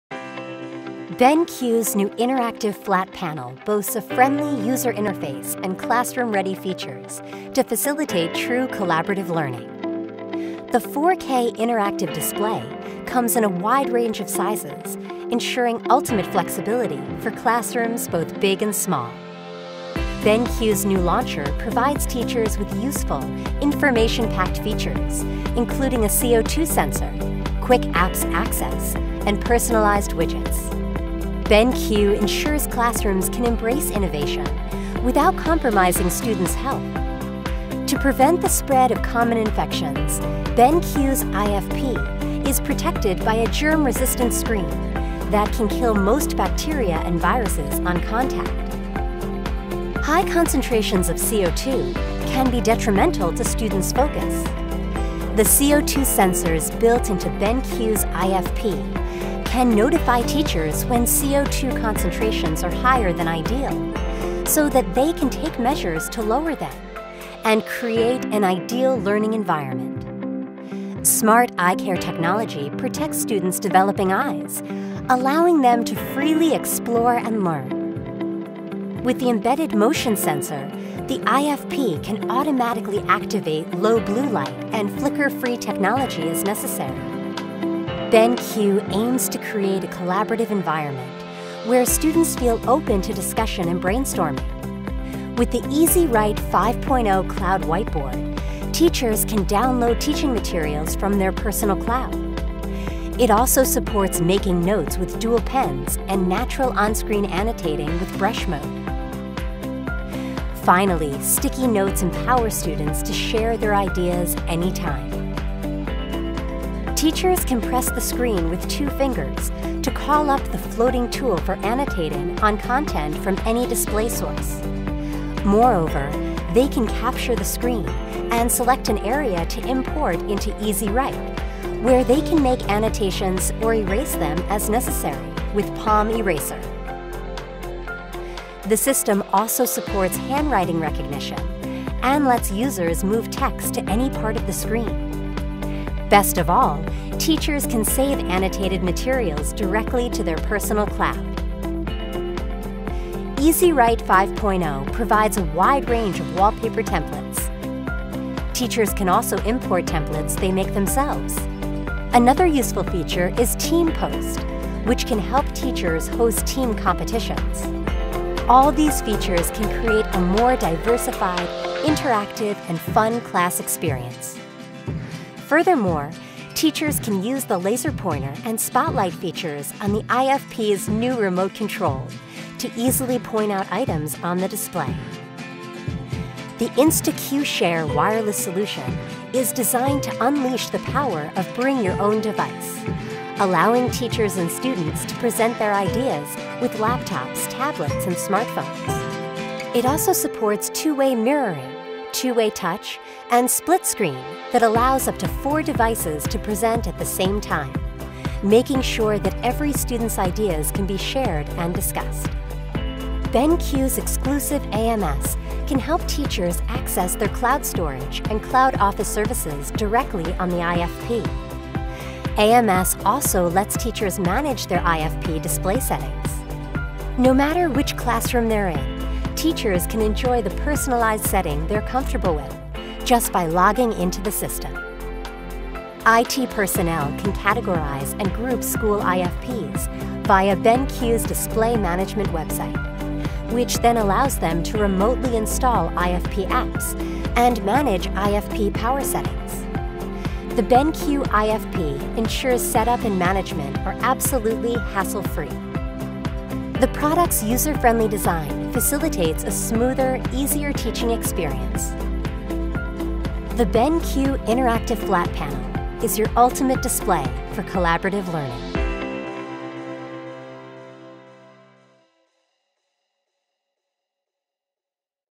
BenQ Education IFP Product Video
Featuring unique ClassroomCare Technologies, the new generation of Germ Resistant and Smart Eye-Care IFP boasts the exclusive Air Quality Sensor, Cloud Whiteboard, a friendly user interface and classroom-ready features to facilitate true collaborative learning.